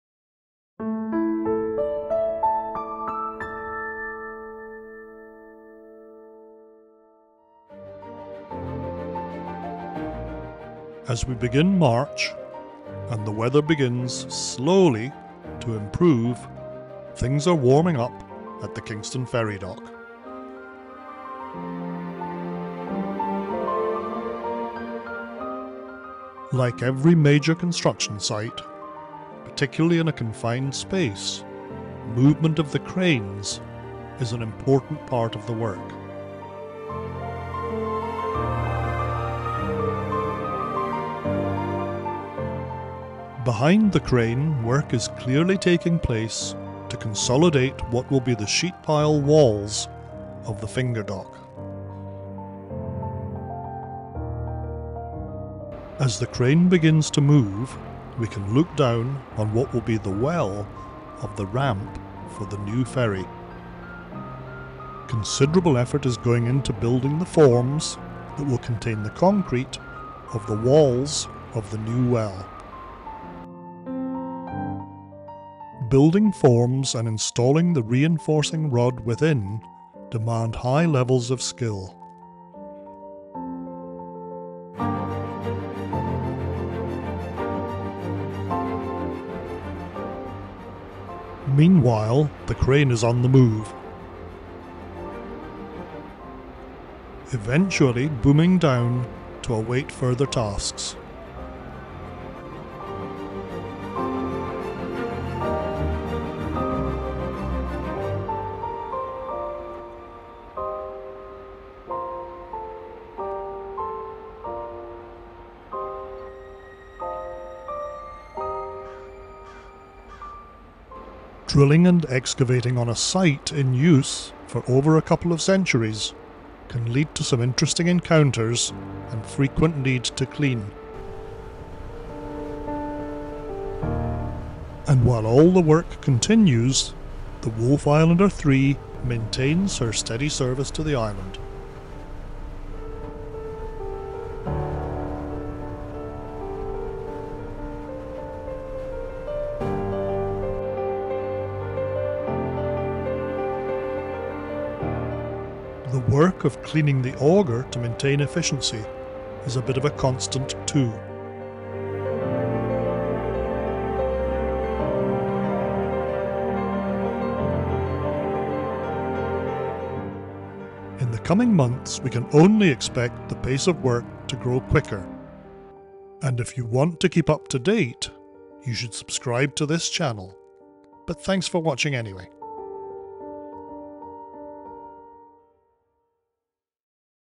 The Pace is Picking up - an update on Kingston's new Ferry Dock 4K
This update sees work taking place to prepare the walls of what will become the ramp well for the new dock. There's also work taking place to complete the end of the finger dock on the south side.

Aerosnapper is a proud member of the Drone Pilot Association of Canada